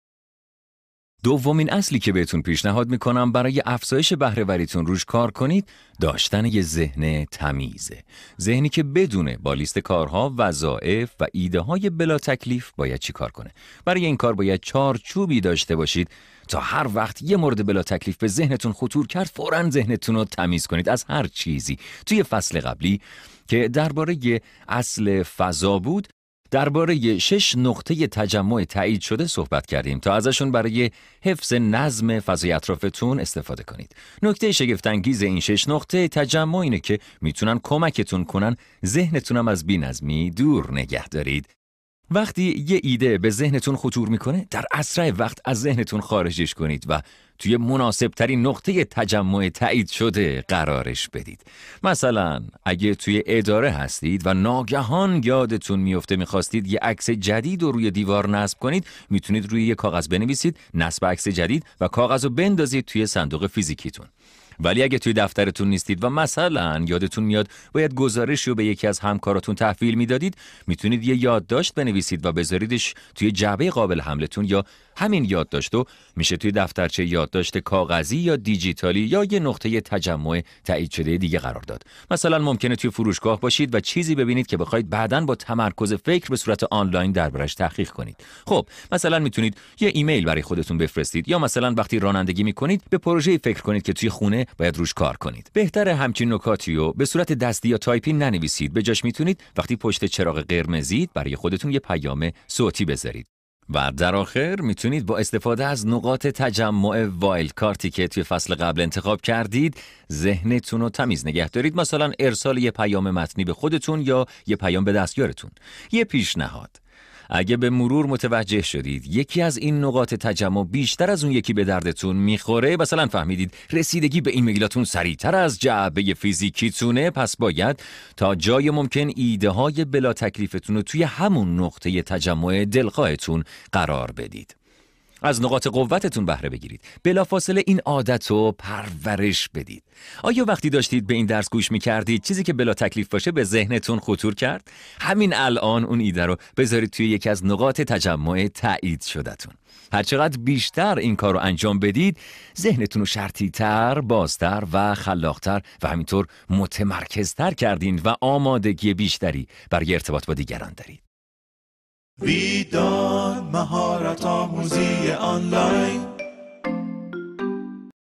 استراتژی موثر برقراری ارتباط | استراتژی های ساده برای پاکسازی ذهن
🔸داو کرانشاو مدرس و مشاور کسب و کار، در دوره "اصول مدیریت زمان" بهره وری ذهن را به شما یاد می‌دهد تا بهره‌وری‌تان در کار بالاتر برود.🔸